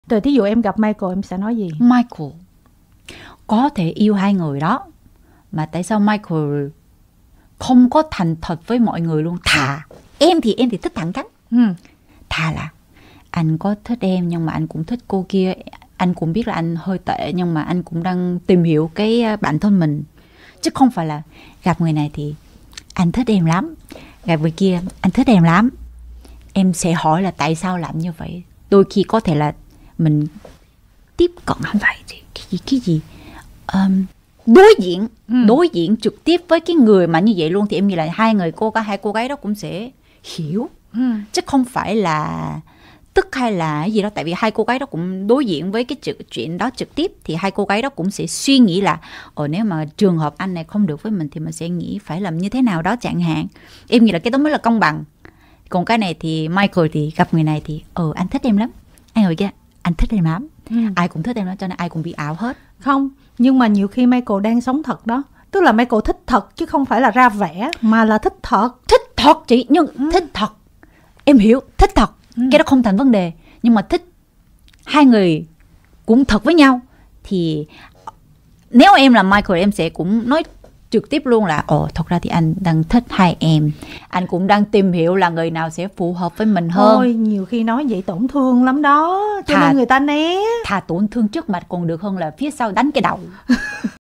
Hari Won: Michael có quyền thích và tìm hiểu với 2 người nhưng nên rõ ràng với họ
Đón xem những tin tức mới nhất hót nhất về Showbiz Việt trên 8 Sài Gòn.

Ca sĩ, MC Hari Won chia sẻ những cảm nhận thú vị với vai trò ban bình luận trong chương trình truyền hình thực tế về hẹn hò đầu tiên Việt Nam kết hợp với Hàn Quốc là Đảo thiên đường.